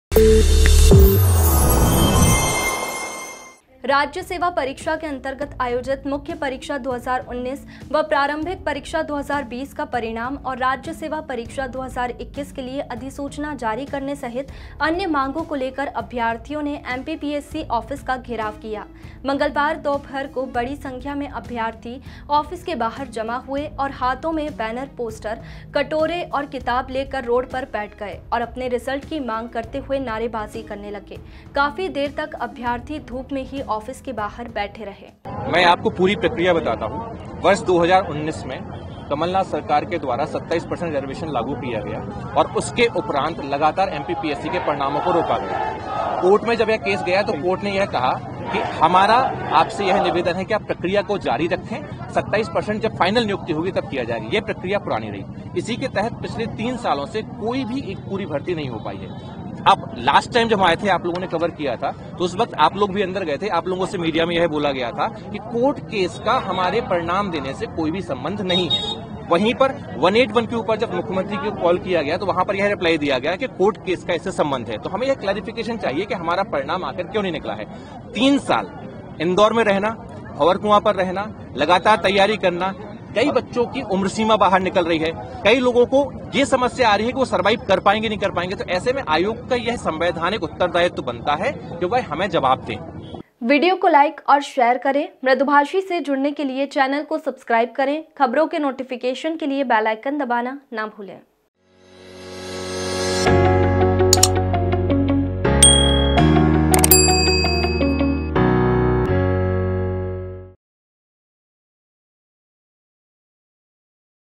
इंदौर में MPPSC ऑफिस का घेराव, कटोरा और किताब लेकर रोड पर बैठे अभ्यर्थी | Mradubhashi
राज्य सेवा परीक्षा के अंतर्गत आयोजित मुख्य परीक्षा 2019 व प्रांरभिक परीक्षा 2020 का परिणाम और राज्य सेवा परीक्षा 2021 के लिए अधिसूचना जारी करने सहित अन्य मांगों को लेकर अभ्यर्थियों ने एमपीपीएससी ऑफिस का घेराव किया। मंगलवार दोपहर को बड़ी संख्या में अभ्यर्थी ऑफिस के बाहर जमा हुए और हाथों में बैनर-पोस्टर, कटोरे और किताब लेकर रोड पर बैठ गए और अपने रिजल्ट की मांग करते